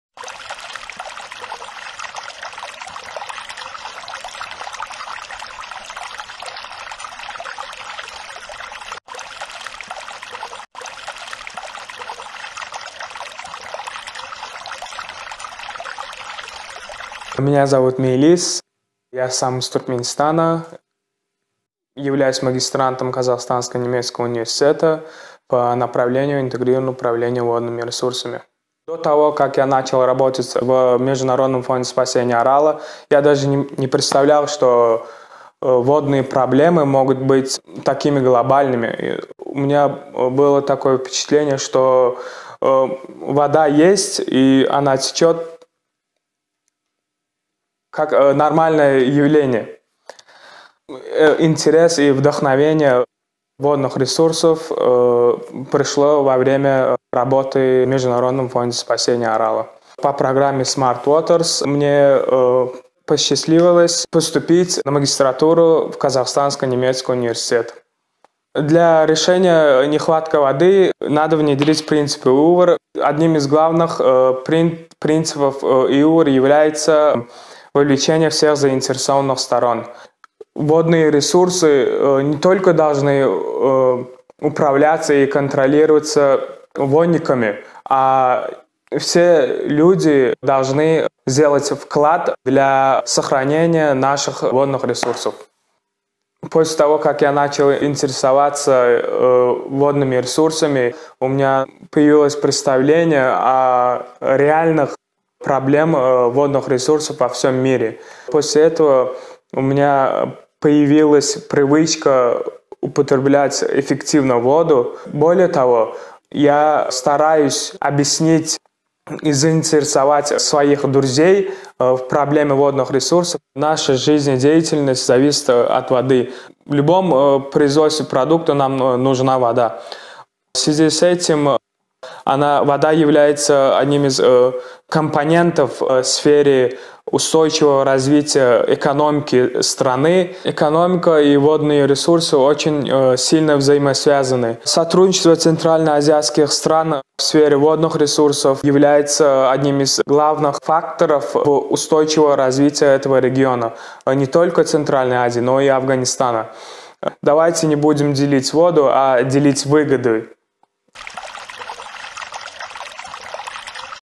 BPCA talks #5, Meylis Amanov
Общая вода - обшие выгоды! Так считает молодой водник из Туркменистана, Мейлис Аманов. Делимся с вами очередным выпуском BPCAtalks!

ENGShared water - shared benefits! Says young water specialist from Turkmenistan, Meylis Amanov. We are glad to share with you the next issue of the BPCAtalks!